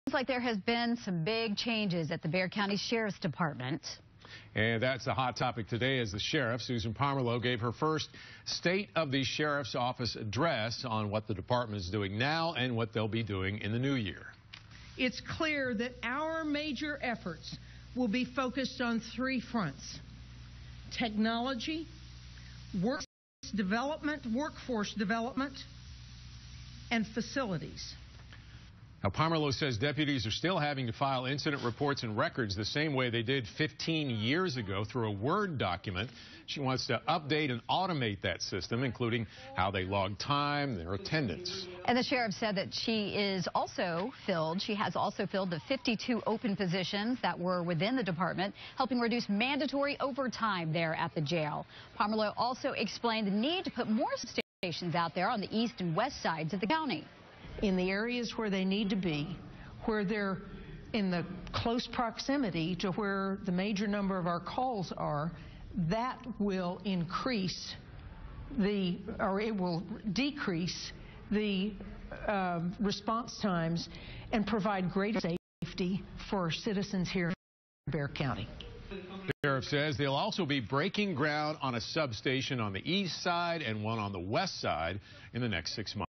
Bexar County Sheriff Office: State of the Sheriff Address
WOAI - Bexar County Sheriff Office: Susan Pamerleau State of the Sheriff Address - Technology, Workforce Development, and Facilities.